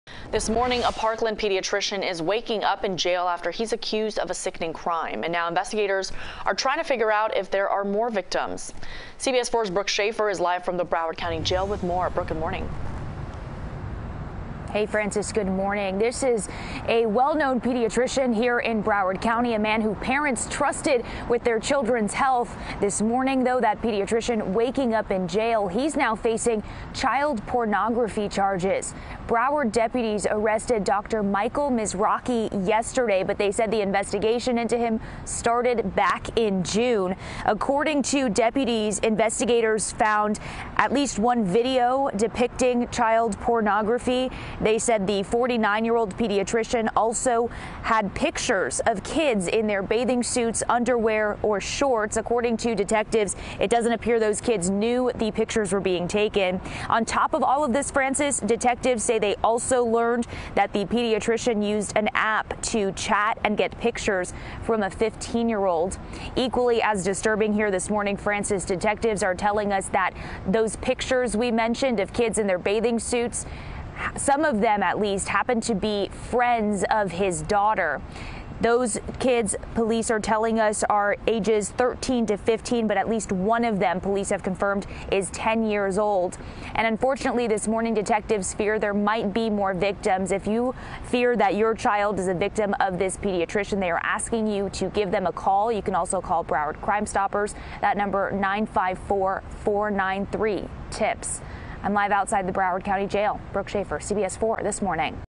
Parkland Pediatrician Facing Child Pornography Charges
Brooke Shafer reports Dr. Michael Mizrachy faces two counts of possession of sexual performance by a child and one count of computer pornography.